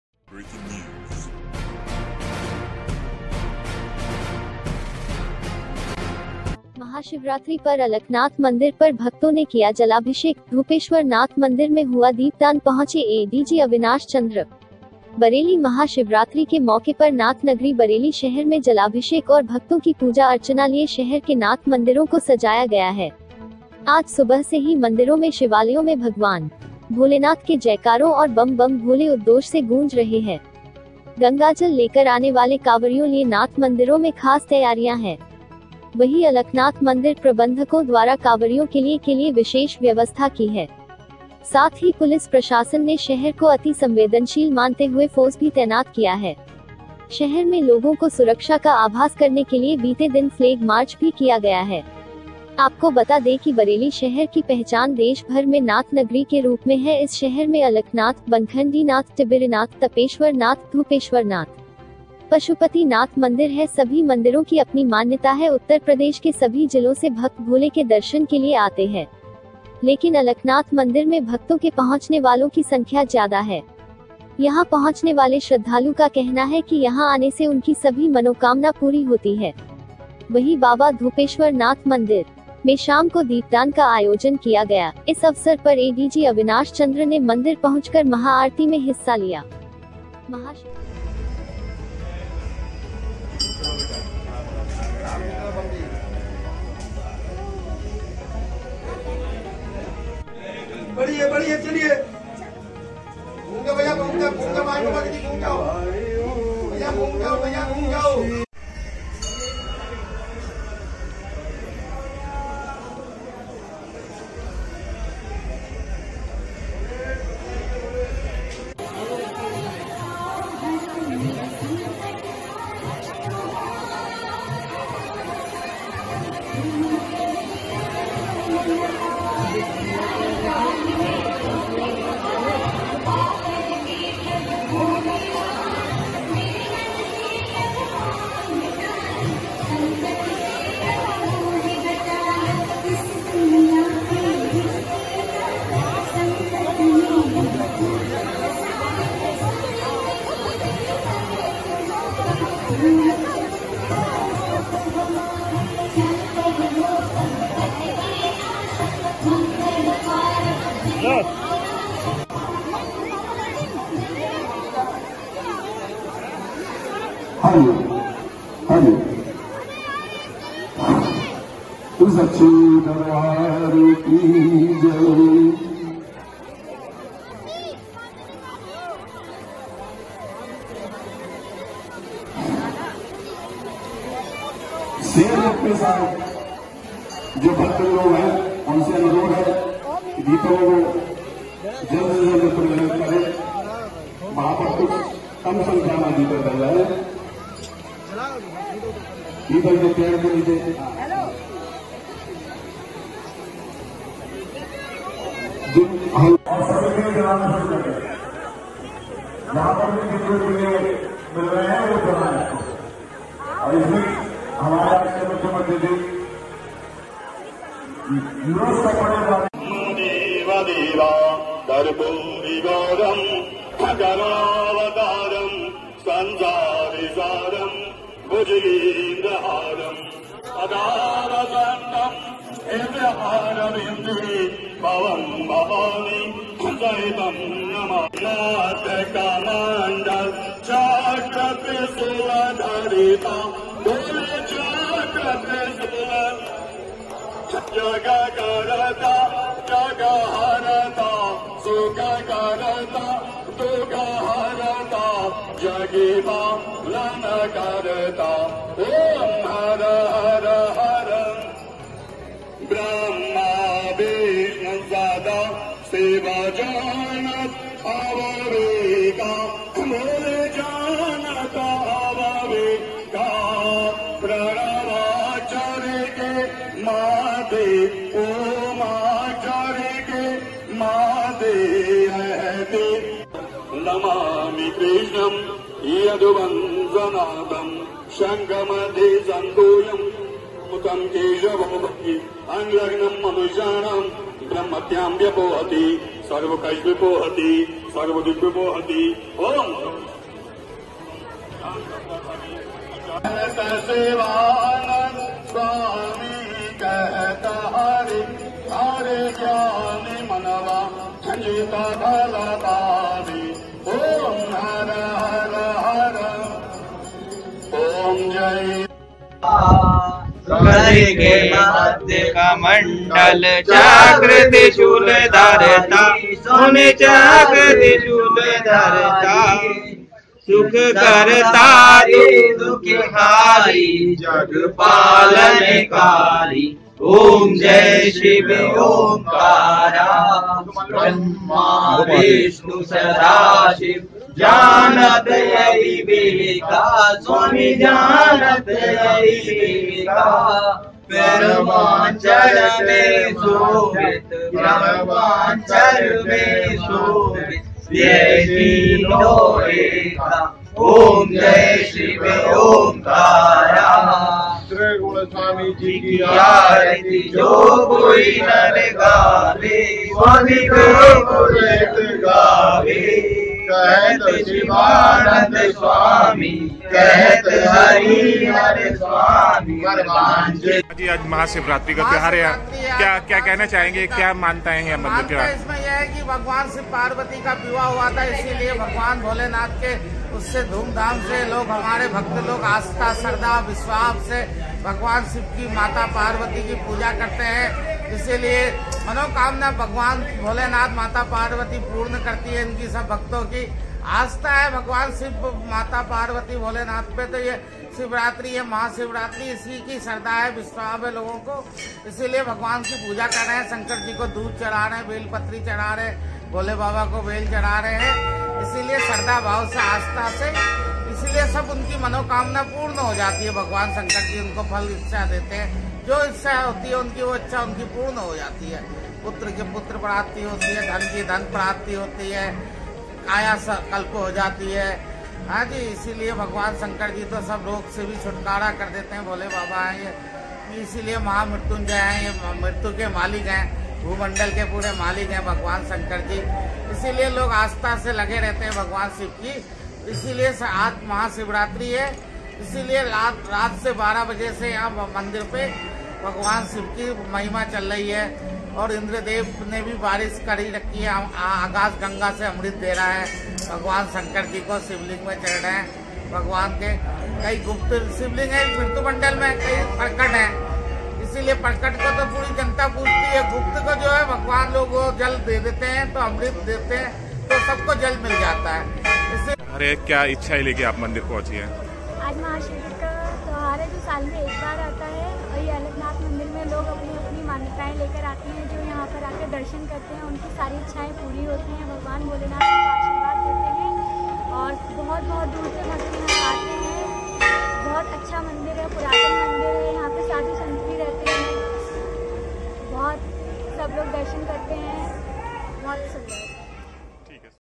महाशिवरात्रि पर अलखनाथ मंदिर पर भक्तों ने किया जलाभिषेक, धोपेश्वर नाथ मंदिर में दीपदान में एडीजी
महाशिवरात्रि पर  अलखनाथ मंदिर पर भक्तों ने किया जलाभिषेक, धोपेश्वर नाथ मंदिर में हुआ दीपदान पहुंचे एडीजी अविनाश चंद्र

बरेली महा शिवरात्रि  के मौके पर नाथ नगरी बरेली शहर में  जलाभिषेक और भक्तों  की  पूजा अर्चना लिए शहर के नाथ मंदिरों को सजाया गया है ।  आज  सुबह से ही मंदिरों में  शिवालयों में  भगवान भोलेनाथ के जयकारों और बम बम भोले  उद्द्दोष से गूंज रहे   है । गंगाजल लेकर आने वाले कावरियों लिए नाथ मंदिरो में खास तैयारियां है । वही अलखनाथ मंदिर प्रबंधकों  द्वारा  कावरियों के लिए के लिए विशेष व्यवस्था  की है. साथ ही पुलिस प्रशासन ने शहर को अति संवेदनशील मानते हुए फ़ोर्स भी तैनात किया है । शहर में लोगों को सुरक्षा का आभास करने के लिए बीते दिन फ्लेग मार्च  भी किया गया है । आपको बता दे कि  बरेली शहर की पहचान देश भर में नाथ नगरी के रूप में है इस शहर में अलखनाथ ,बनखंडीनाथ , टिबिरिनाथ ,तपेश्वर नाथ ,धोपेश्वरनाथ ,पशुपति नाथ मंदिर है सभी मंदिरों की अपनी मान्यता है उत्तर प्रदेश के सभी जिलों से भक्त  भोले के दर्शन के लिए आते है । लेकिन  अलखनाथ मंदिर में भक्तों के पहुंचने वालों की संख्या ज्यादा है। यहां पहुंचने वाले श्रद्धालु का कहना है कि यहां आने से उनकी सभी मनोकामना पूरी होती है। वही बाबा धोपेश्वर नाथ मंदिर में शाम को दीपदान का आयोजन किया गया,  इस अवसर पर एडीजी अविनाश चंद्र ने मंदिर पहुंचकर महा आरती में हिस्सा लिया